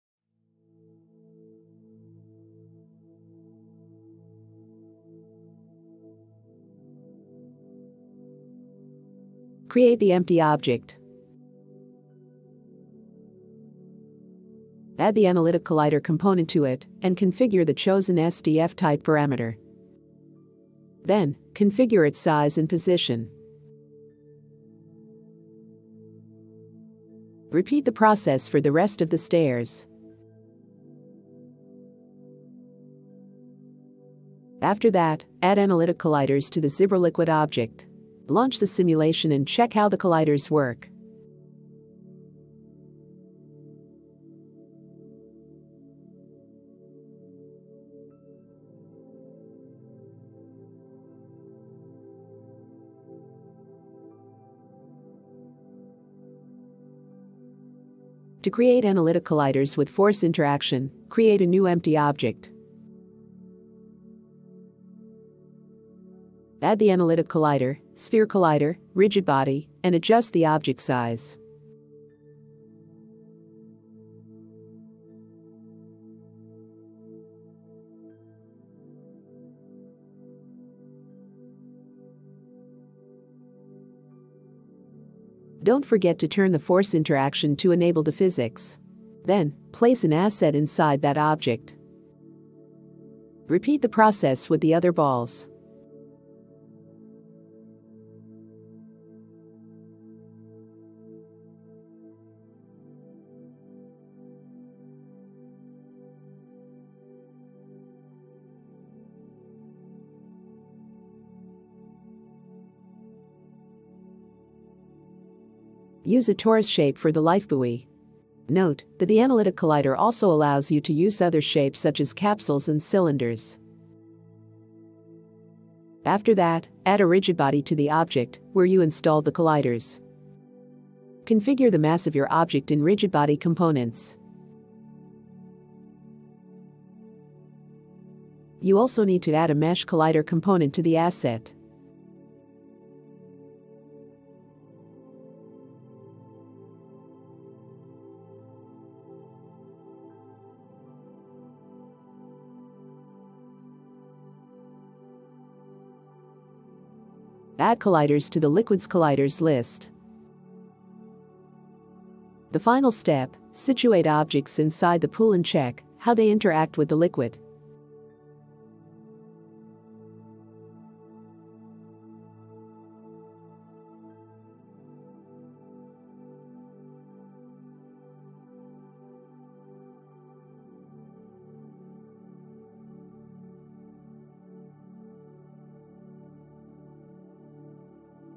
Zibra Liquids - Tutorial 5. How to create analytic colliders? | Unity 3D
We have covered the basics, so it’s time for some really interesting stuff, liquid colliders.
There are several types of them in Zibra Liquids.This time, we will stop at analytic colliders, which allow you to add simple geometry, such as spheres, boxes, capsules, toruses, and cylinders. In this tutorial, we will show you how to create them. We will also cover how to make these colliders force interactive.

00:00 How to create analytic colliders
00:59 How to create analytic colliders with force interaction

Don’t forget, If you have any additional questions, we would be happy to answer them on our Discord.

System requirements

Editor platforms: PC (DX11, x86 and x64), macOS(Metal, x64 and arm64) For M1 Mac users, the M1 native version of Unity Editor is strongly recommended.
Unity version: 2019.4 or later (use the latest feature update)
Supported Render Pipelines: SRP, URP, HDRP
URP: 7.5 or later
HDRP: 7.x or later